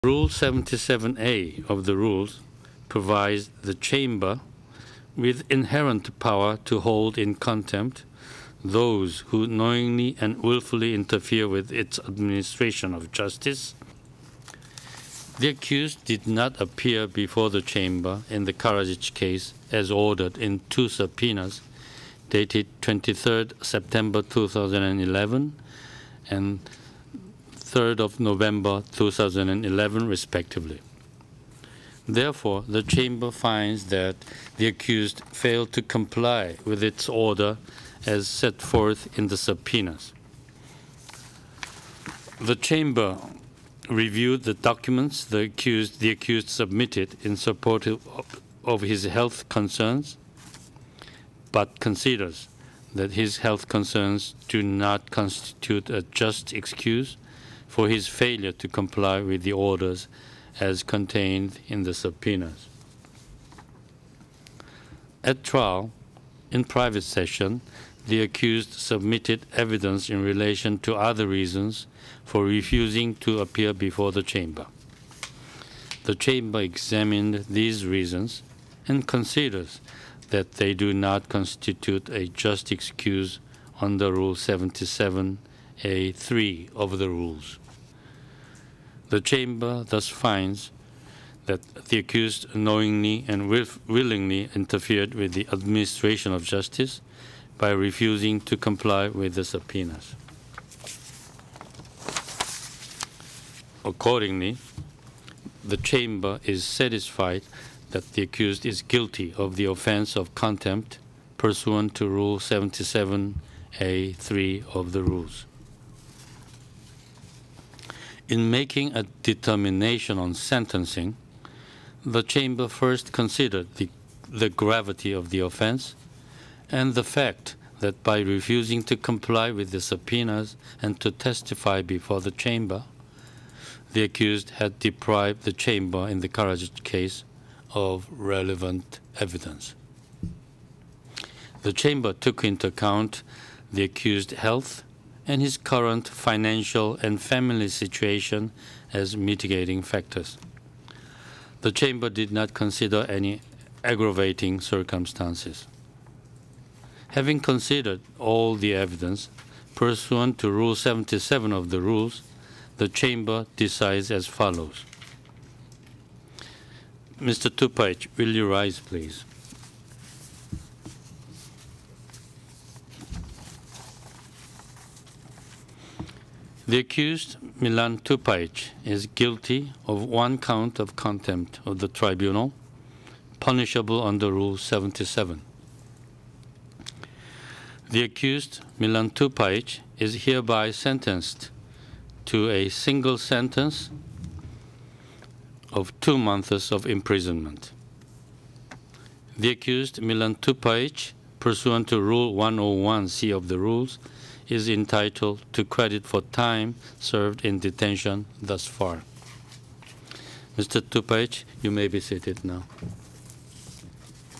Judgement - Tupajić (Contempt) - 24 February 2012
Today Trial Chamber III sentenced Milan Tupajić, former chief of the crisis staff and war-time president of the municipality of Sokolac, Bosnia and Herzegovina, to 2 months' imprisonment for failing to comply with the Chamber's orders to appear as a witness in the trial of Radovan Karadžić.

In rendering its Judgement, the Chamber "considered the gravity of the offence and the fact that by refusing to comply with the subpoenas and to testify before the Chamber, the Accused had deprived the Chamber in the Karadžić case of relevant evidence."

The Trial Chamber first considered Tupajić's claim that his primary reason for refusing to appear before the Chamber related to his health concerns. After reviewing documents in support of his health concerns, the Chamber considered that "his health concerns do not constitute a just excuse for his failure to comply with the orders as contained in the subpoenas."

The Chamber further found that Tupajić was aware of the contents of the subpoenas and the obligation contained therein, as well as of the consequences of his failure to comply with the subpoenas.

Tupajić is entitled to credit for time served in detention thus far.

The Order in Lieu of Indictment was filed on 30 November 2011. Tupajić was arrested by Bosnian authorities on Tuesday, 13 December 2011. At his initial appearance on 16 December 2011, Tupajić pleaded not guilty. The trial was held on 3 February 2012.